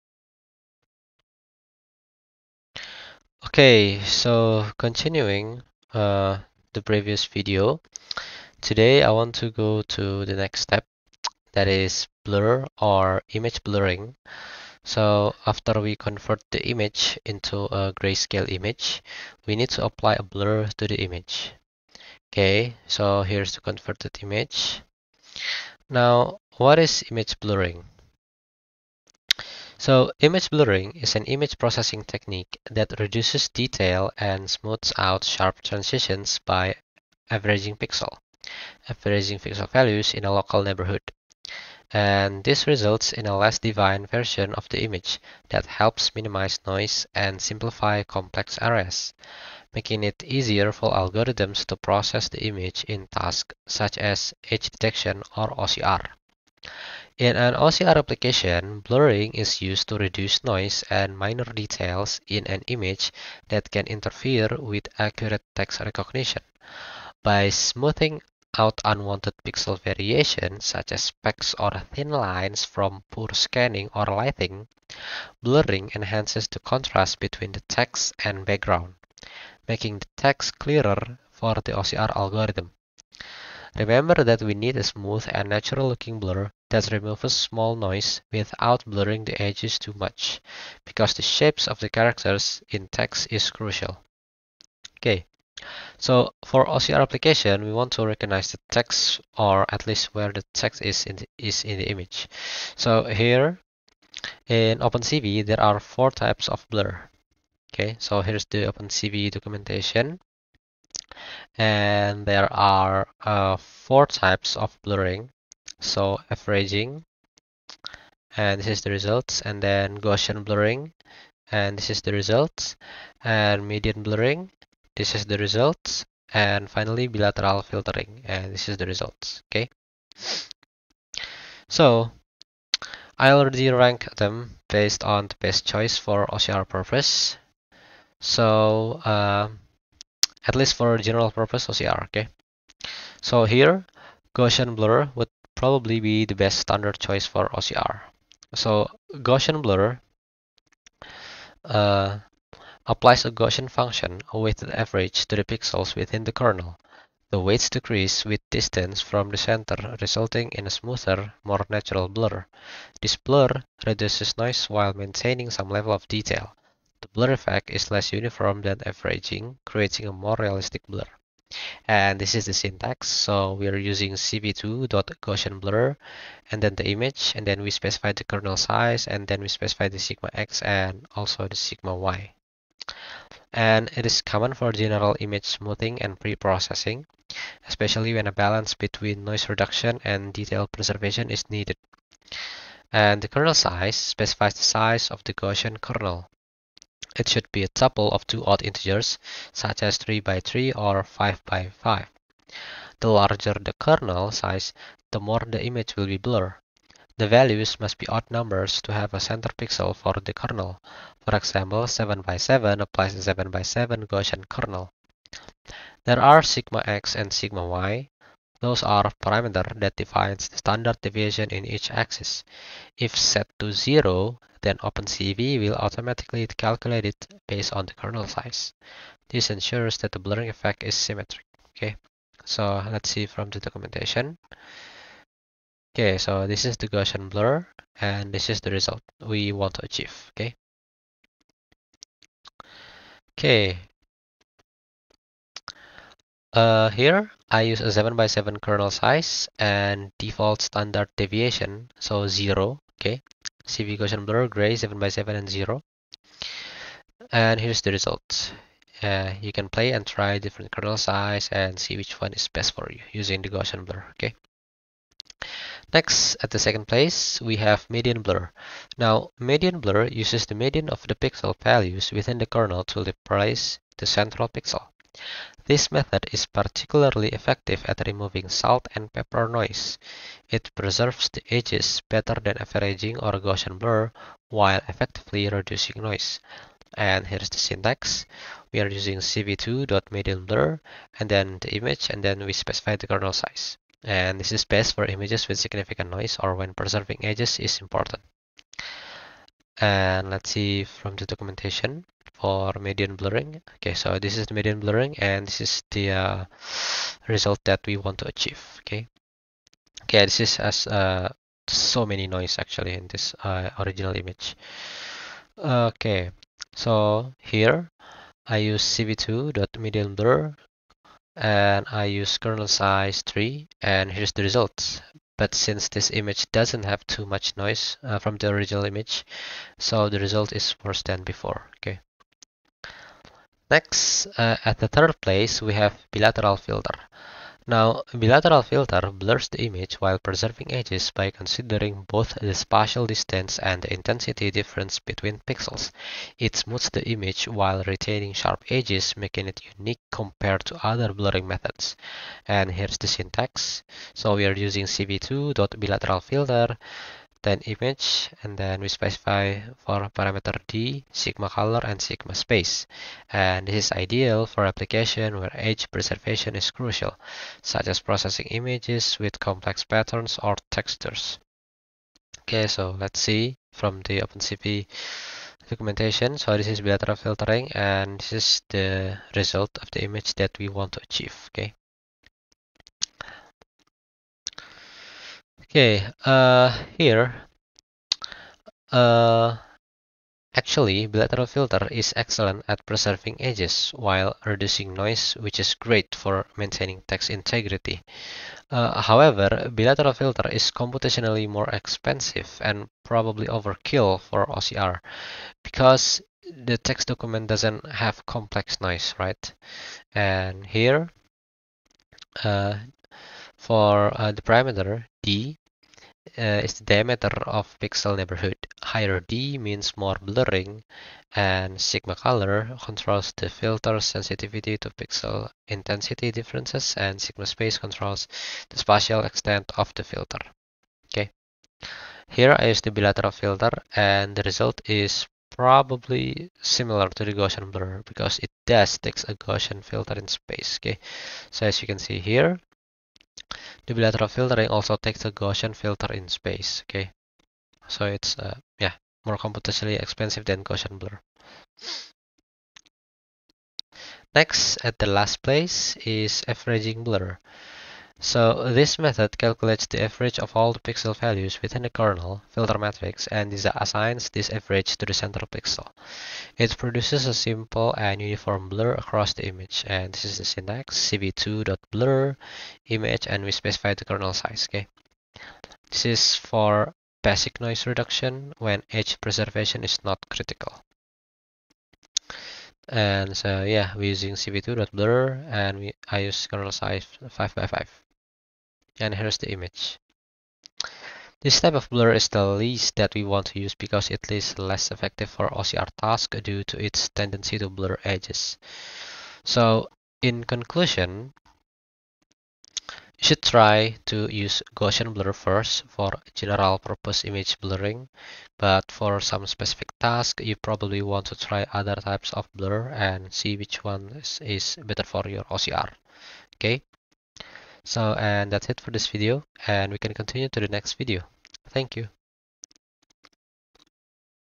2. How to Apply Image Blurring for OCR Pre-processing (EasyOCR + OpenCV)
In this video, we continue our OCR programming series by exploring image blurring techniques in Python with OpenCV. Blurring is a vital preprocessing step that smooths images, reduces noise, and enhances text visibility, making it an essential tool for Optical Character Recognition (OCR).

In this tutorial, we cover:

1. An overview of blurring techniques, including Gaussian, Median, Bilateral, and Averaging blur, ranked based on their effectiveness for OCR.

2. How each blur method works, with a focus on Gaussian blur as the most suitable option for general OCR tasks.

3. Practical guidance on adjusting parameters such as kernel size and filter sensitivity to achieve the best results for your specific OCR needs.

This video will help you understand the role of image blurring in OCR and how to optimize it to ensure clearer text recognition. Whether you’re working on document scanning, text extraction, or simply improving OCR performance, these techniques will make your workflow more efficient.